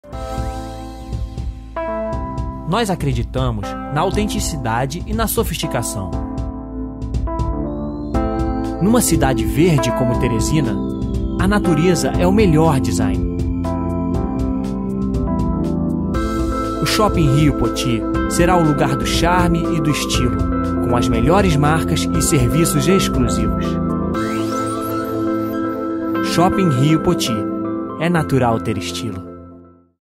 VT Rio Poty
Uma pequena amostra do que será a Reserva Rio Poty. Shopping, parque, reserva. Um novo conceito em empreendimento multiuso.